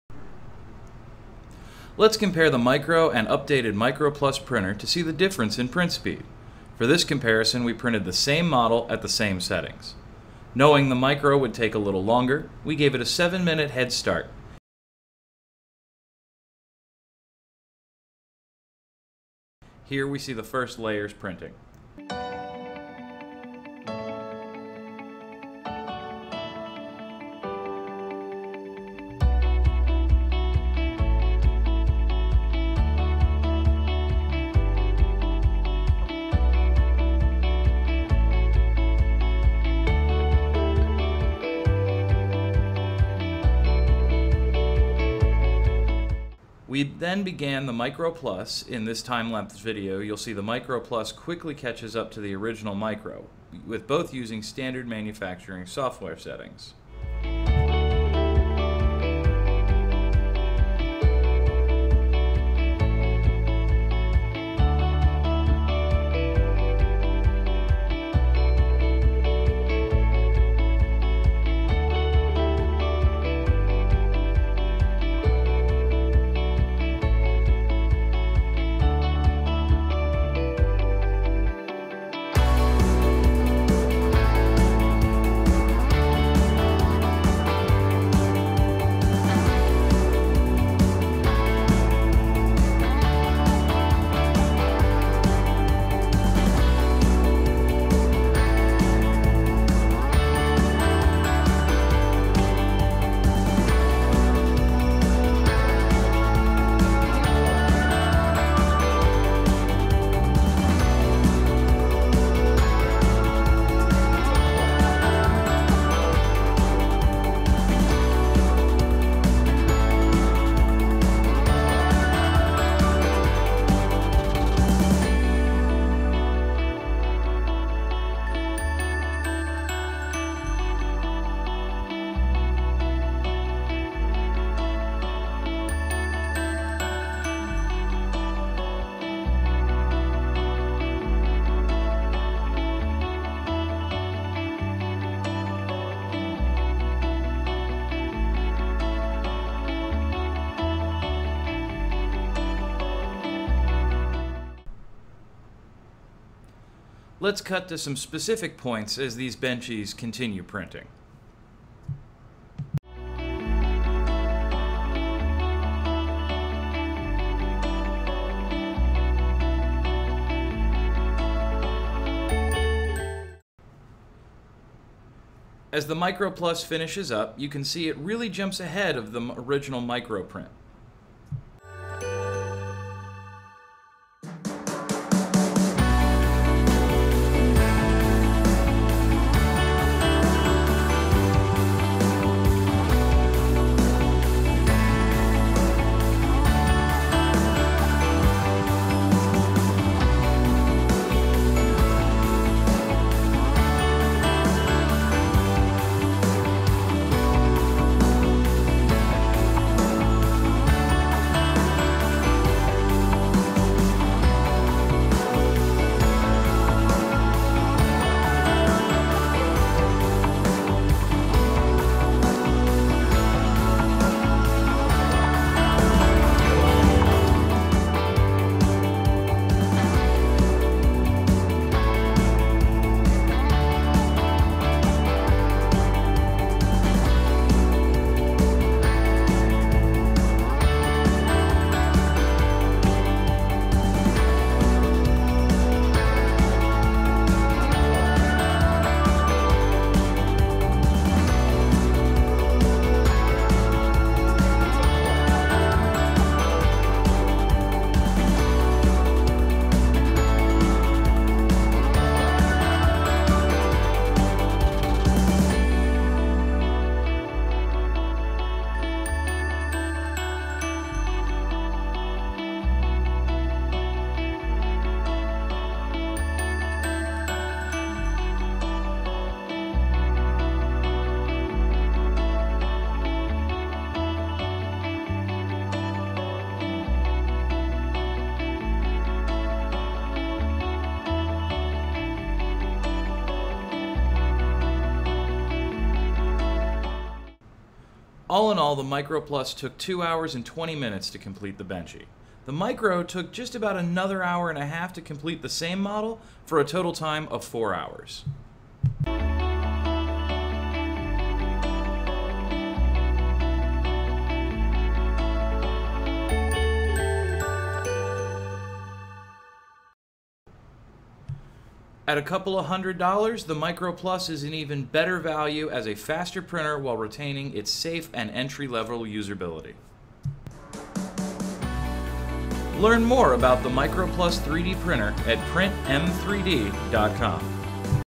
M3D's Micro - M+ 3D Printer Race
In this time lapsed video, see one of the components updated on the Micro+: speed. The Micro+ can be purchased on our site, in addition to an upgrade kit that customers can purchase to swap out key components on their Micro to have all of the upgrade and advantages of the Micro+. printm3d.com